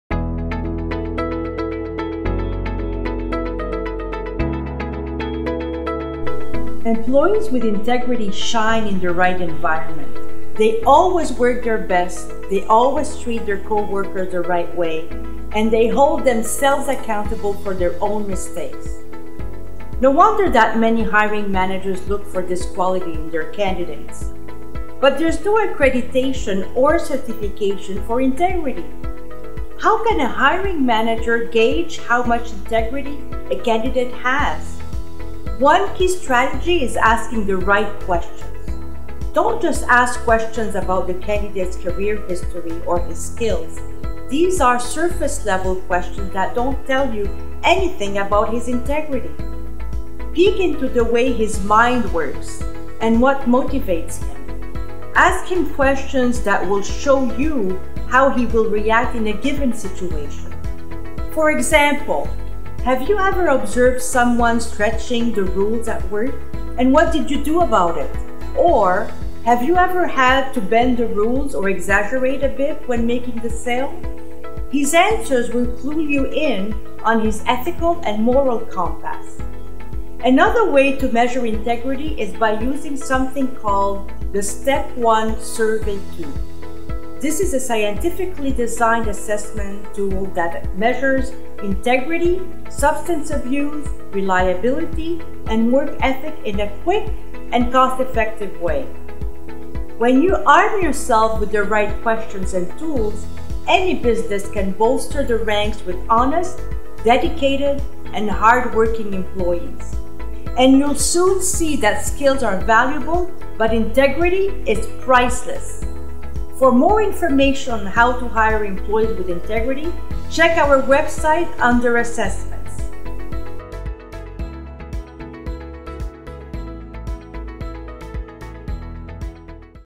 How Can You Measure Candidate's Integrity?
Employees with integrity shine in the right environment. No wonder that many hiring managers look for this quality in their candidates.Having the right skills is valuable but integrity is priceless! Listen to this video and learn how you can measure integrity.

where you will find valuable information for your small business. Receive a copy of Aline Ayoub's book Hire The Right Fit.